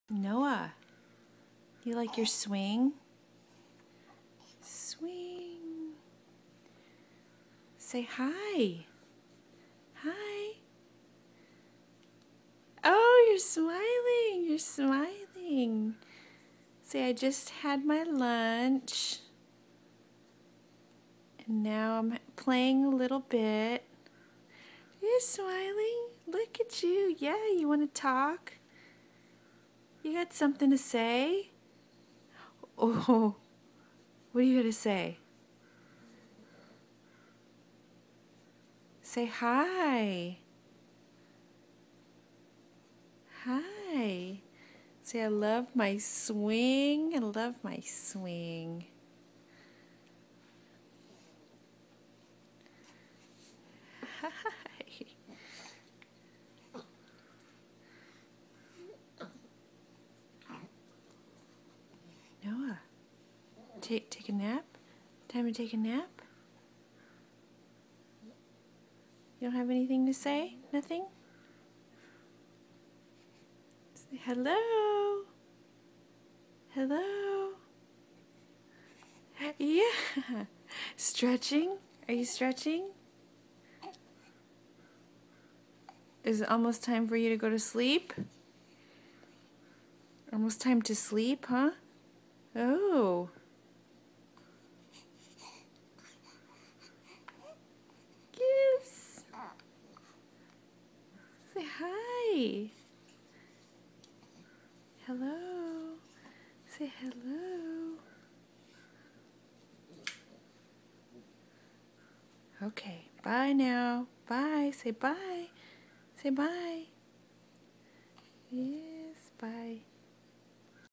Noah - 1 month old
Hanging out in his swing after lunch